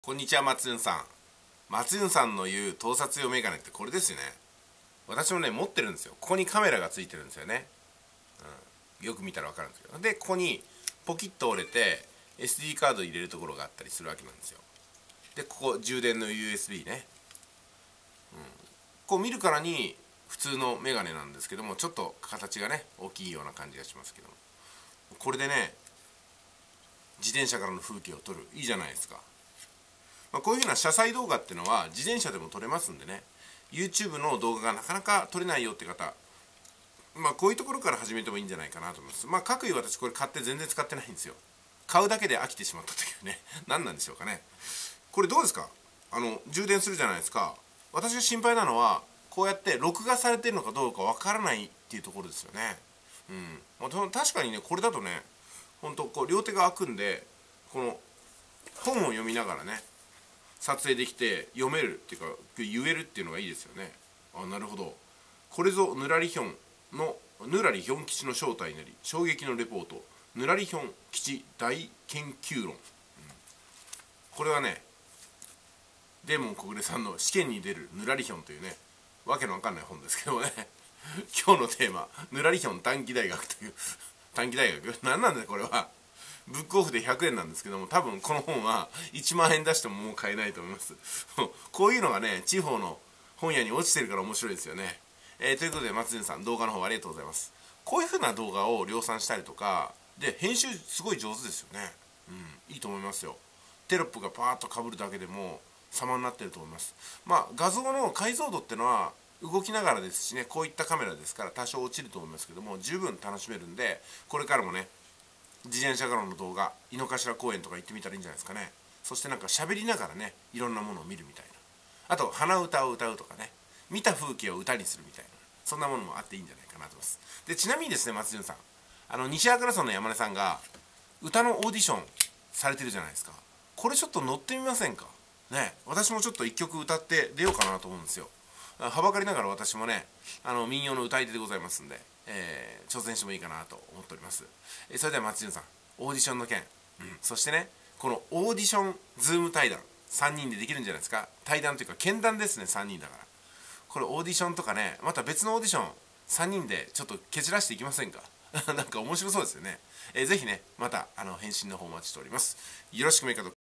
【隠居TV】松じゅんさんへ「よろしくメカドック」
お疲れ様です、久保さん。
先日の無料レポートの動画は2月までじゃなく、2月末日までに完成させるとの訂正動画をアップしました。
また今回は盗撮用メガネで撮った風景をアップしました。朝の通勤風景の一部になります。

「件名：速稼塾（あなたのお名前）」